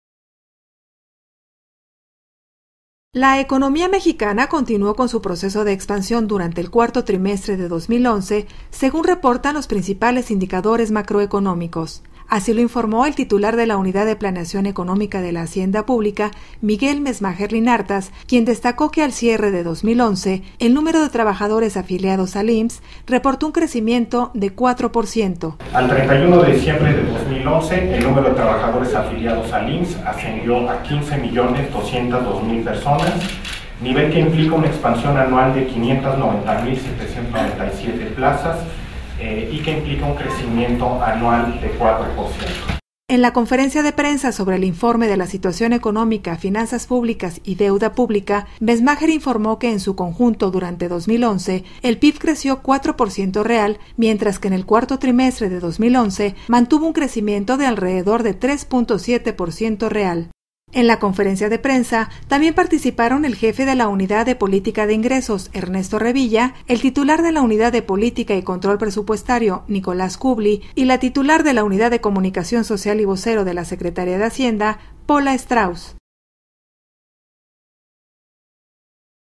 México continuó con su proceso de expansión económica en el cuarto trimestre de 2011
La economía mexicana continuó con su proceso de expansión durante el cuarto trimestre de 2011, según reportan los principales indicadores macroeconómicos. Así lo informó el Titular de la Unidad de Planeación Económica de la Hacienda Pública, Miguel Messmacher Linartas, quien destacó que al cierre de 2011, el número de trabajadores afiliados al IMSS reportó un crecimiento de 4 por ciento.